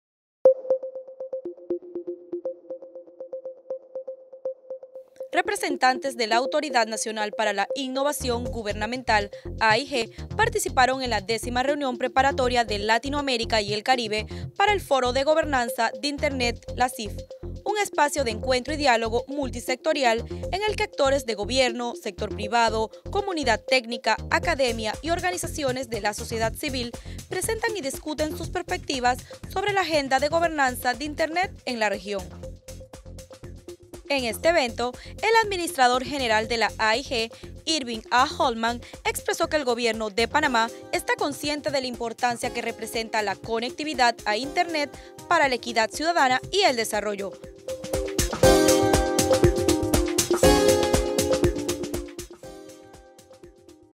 Panamá impulsa conectividad a Internet para la equidad ciudadana
Al inaugurar la Décima Reunión Preparatoria de Latinoamérica y el Caribe para el Foro de Gobernanza de Internet (LACIGF), el administrador general de la Autoridad Nacional para la Innovación Gubernamental (AIG), Irvin A. Halman, expresó que el Gobierno de Panamá está consciente de la importancia que representa la conectividad a Internet para la equidad ciudadana y el desarrollo; por ello, iniciará la ampliación de la nueva Red Nacional de Internet 2.0 “Internet para todos”, con múltiples beneficios.